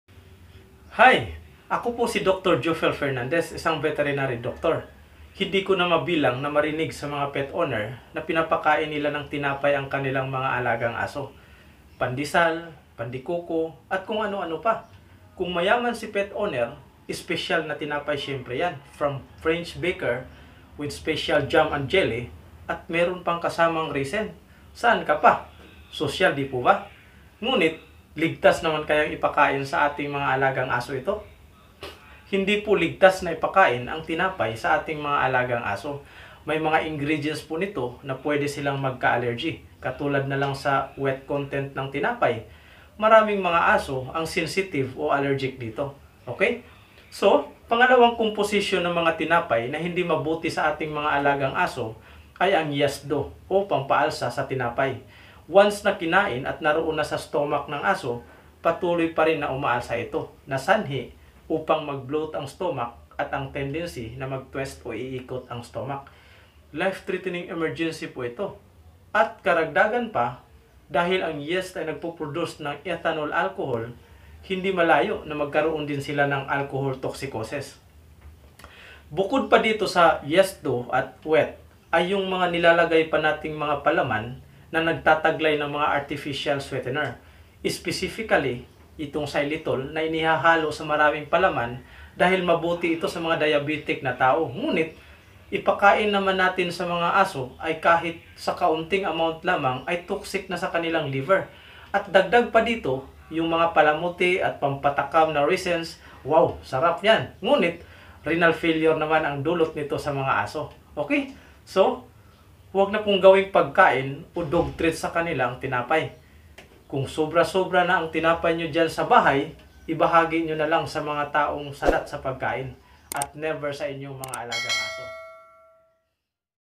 Harmful Food for Dogs and Cats : Ligtas ba Ipakain ang Tinapay sa mga Aso? (series 2)
Toxic food for dogs and cats.is bread safe to eat by our dogs?This video will give awareness and reminders to pet owner.That some of the human foods are harmful to pets.And one should be careful on giving extra foods to them.